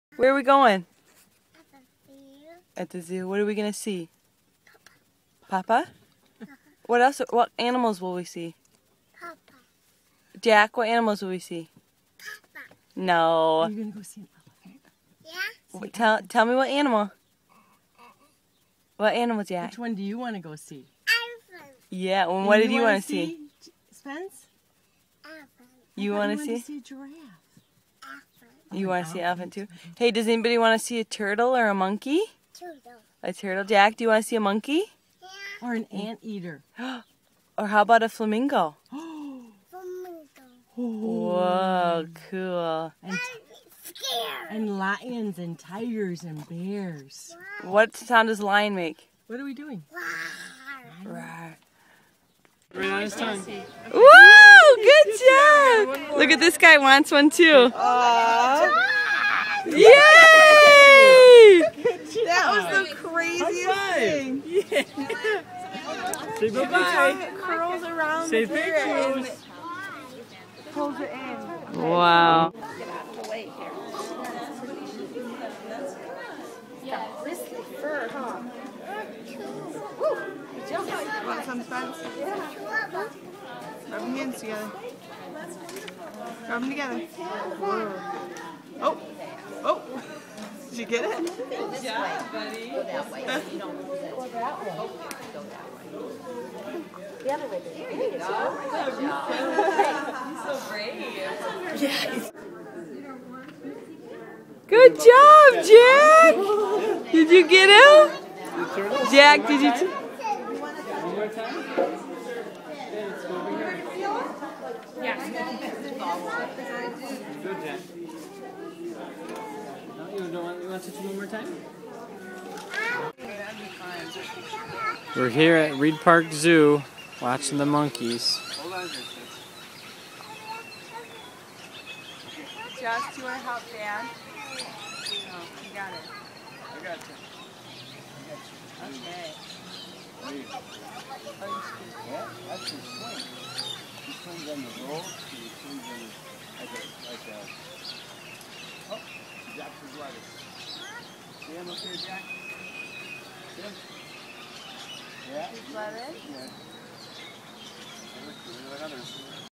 Reid Park Zoo - 2011
Taking Spencer, Jackson, Abigail to Tucson Zoo.  What a fun trip to the zoo with Papa and Nana Horses.